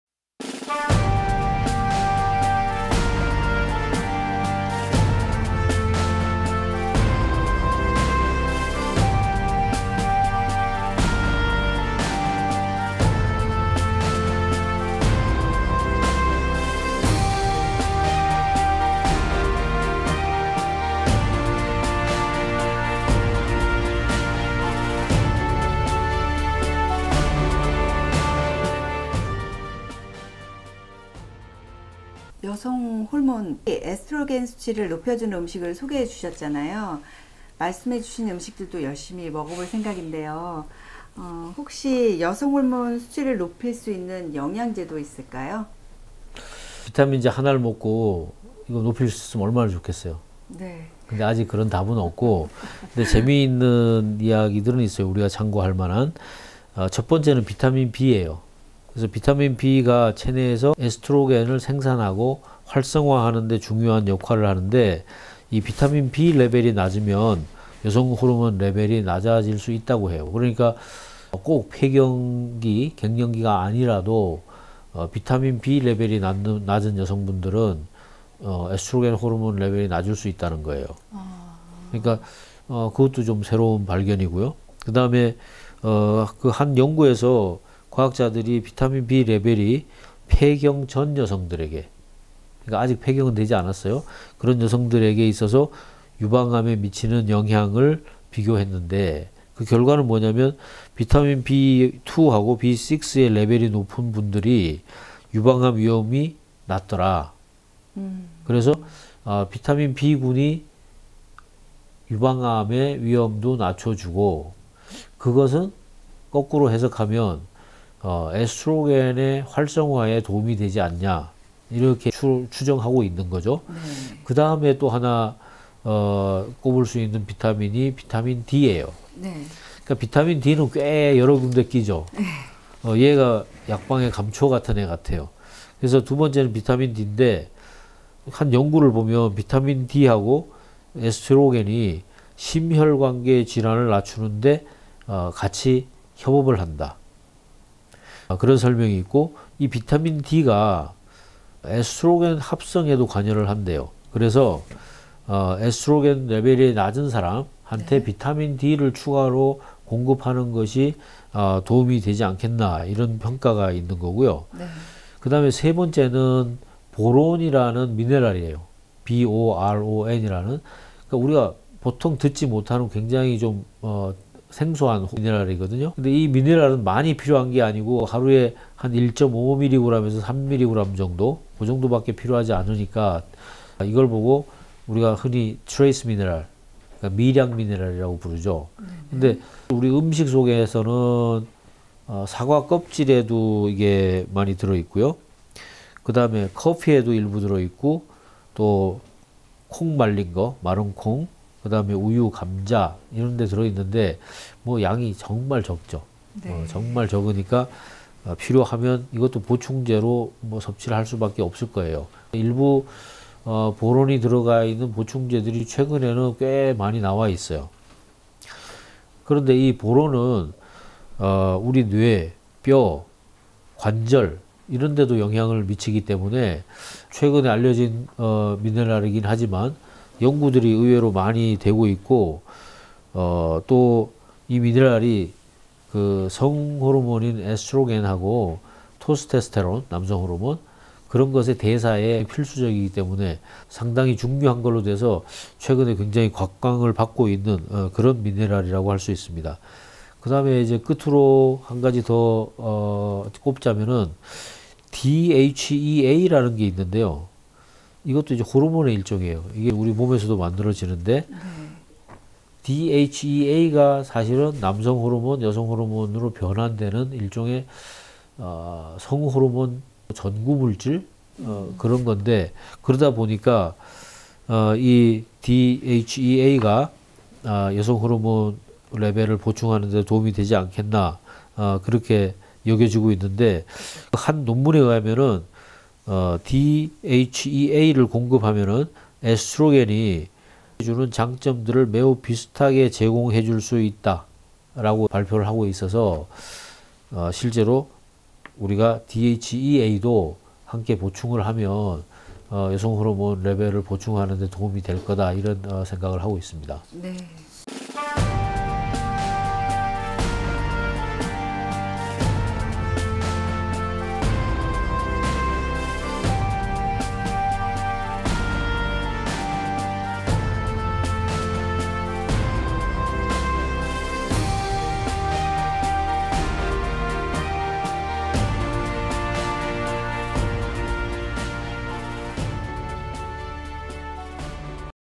[E735] 여성호르몬을 만들 때 꼭 필요한 4가지 건강식품 - 골다공증 퇴치를 위한 '뼈건강 바이블' 제26부'
여성호르몬을 만들 때 꼭 필요한 4가지 건강식품
골다공증 퇴치를 위한 '뼈건강 바이블' 제26부'에서는 여성호르몬 생성에 도움을 주는 4가지의 건강식품을 알려드립니다.

어메이징 이온 칼맥이나 바이탈 멀티, 혹은 MJ101, 레드 오메가-3, 유산균, 호베니아-Rx, 인슈바나를 구입하시려면